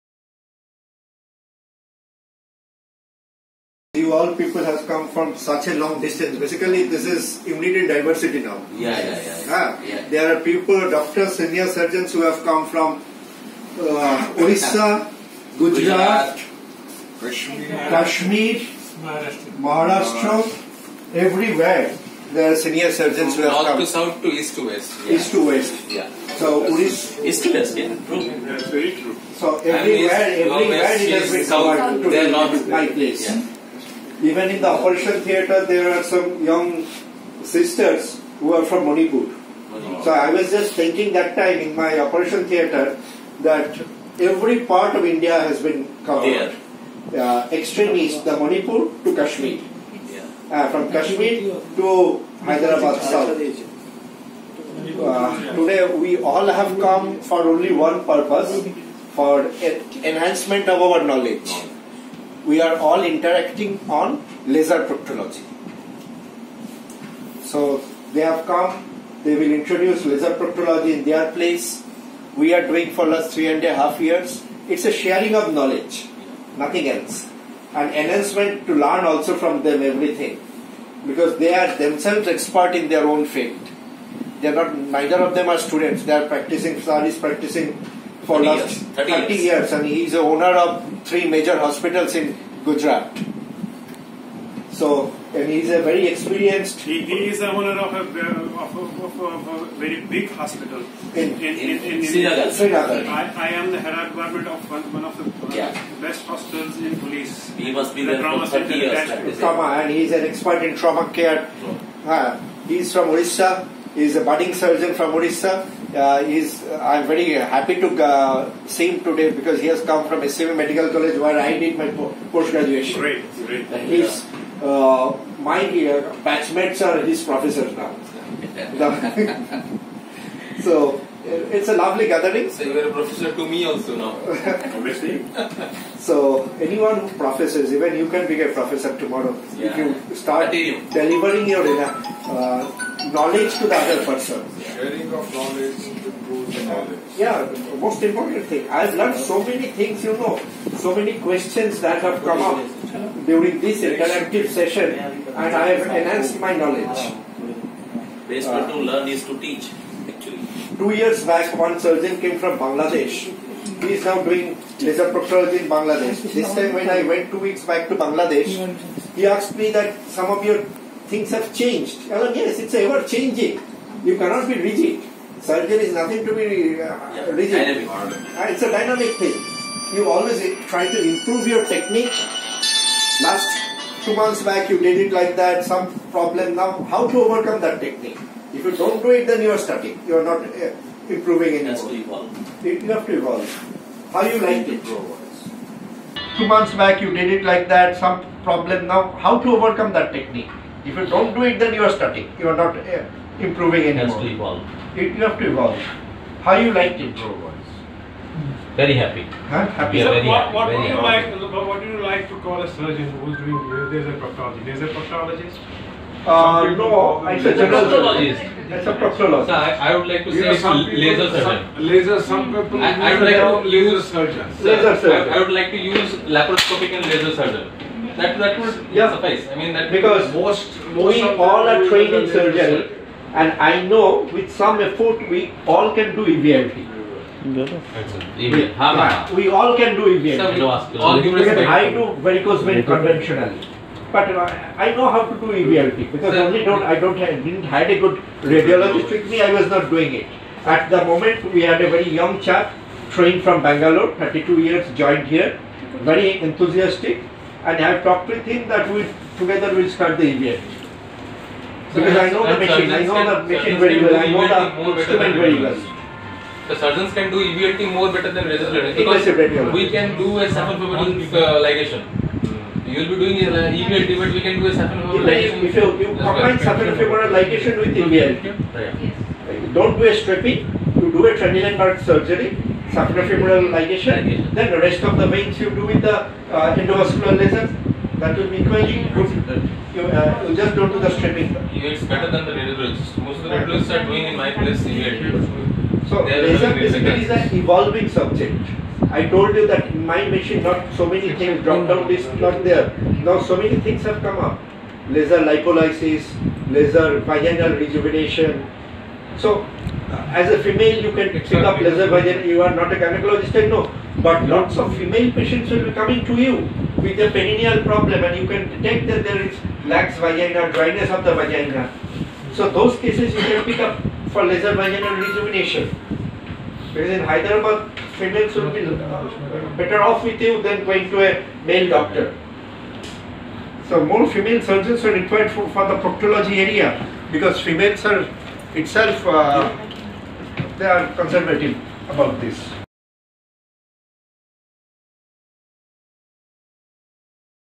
Dr Kalyan Kar delivers a talk on laser practology
Dr Kalyan Kar, MS FRCS (EDIN), Head of the Department Surgery delivers a talk on laser practology. Health practitioners from various state of India are present, as the knowledge-sharing session is conducted at AMRI Hospitals Mukundapur.